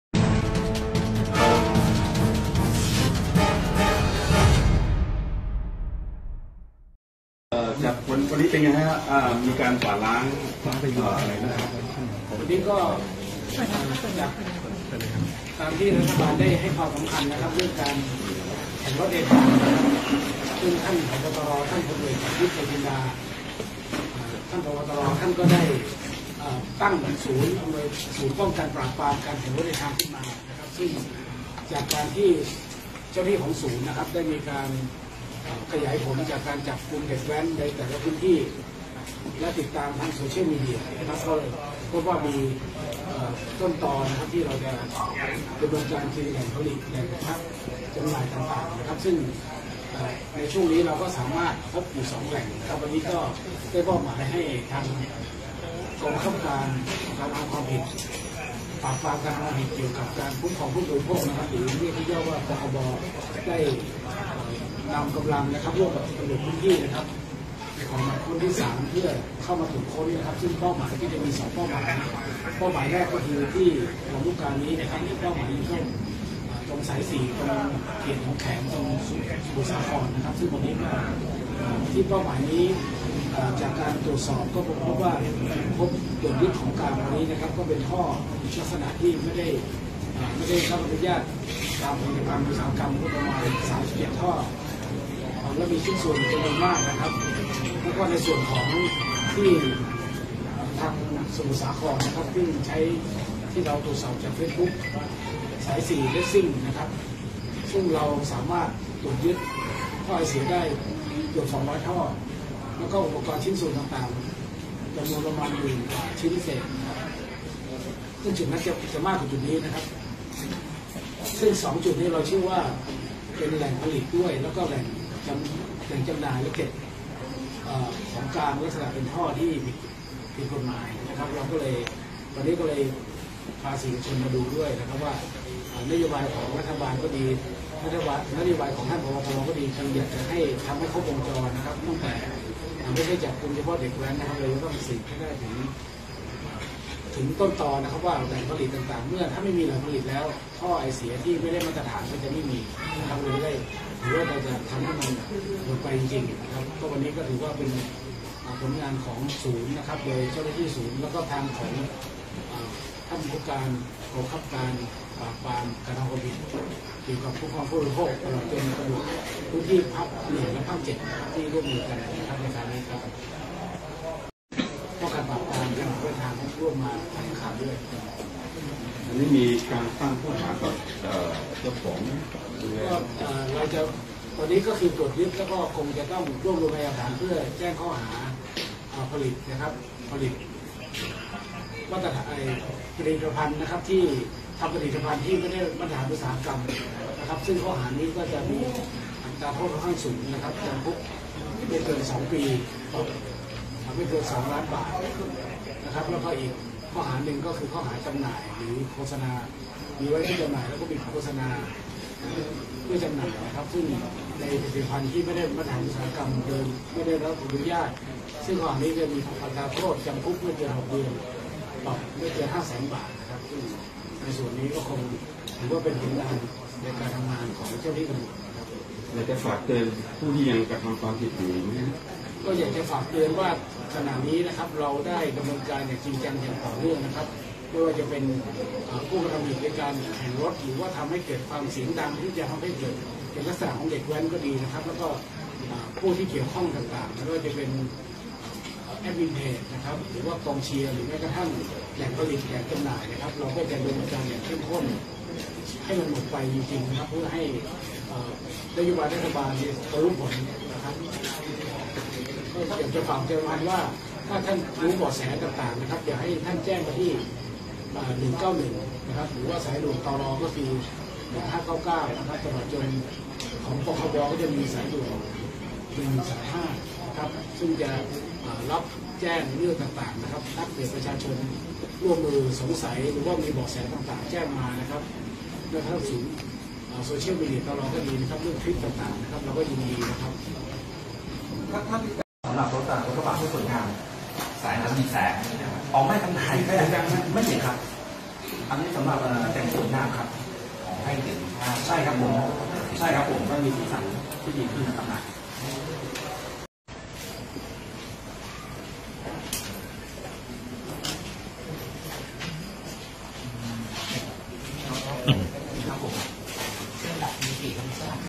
ตร.บุกจับลักลอบผลิตและจําหน่ายท่อไอเสียรถจยย.ไม่ได้มาตรฐาน ย่านลําลูกกา - หนองแขม
ตร.บุกจับลักลอบผลิตและจําหน่ายท่อไอเสียรถจยย.ไม่ได้มาตรฐาน ย่านลําลูกกา - หนองแขม ยึดของกลางนับหมื่นชิ้น 18/11/2019